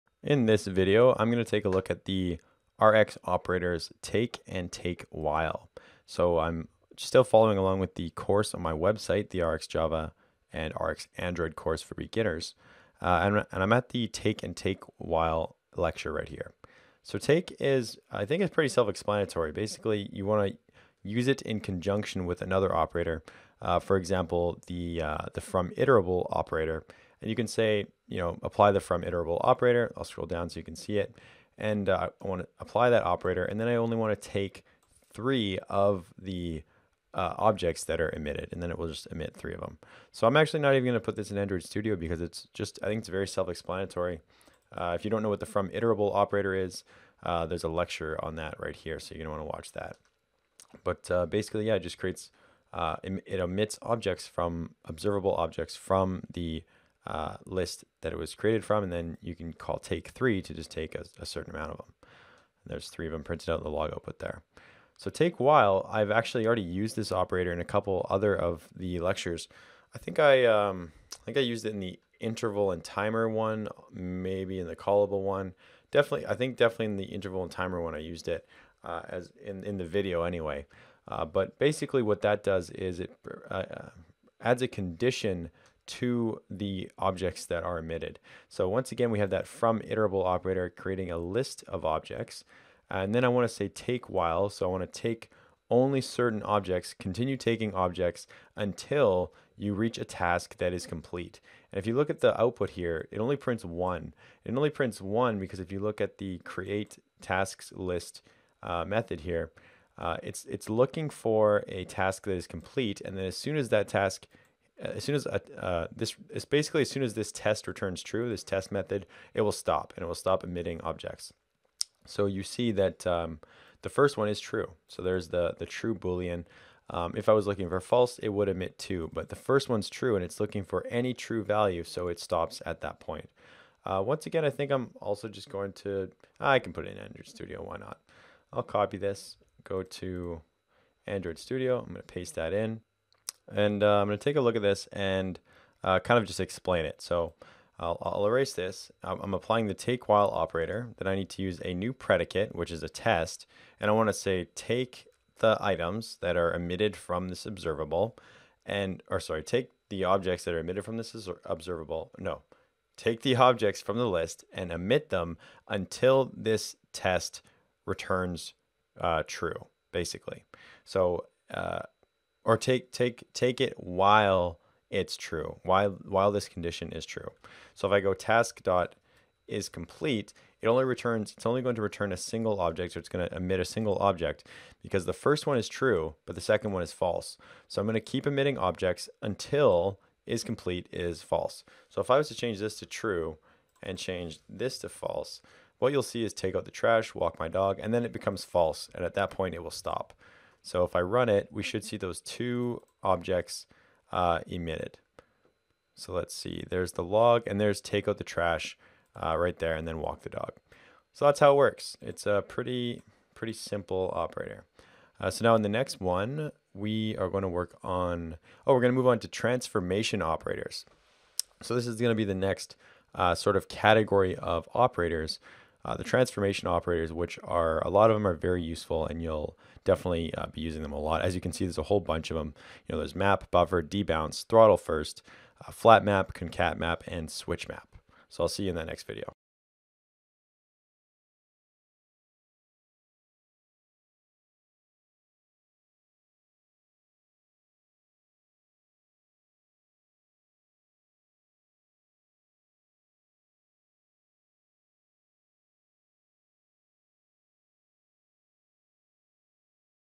Take and TakeWhile Operators - RxJava
In this video I show you how to use the Take and TakeWhile operators to filter the objects emitted by Observables in RxJava.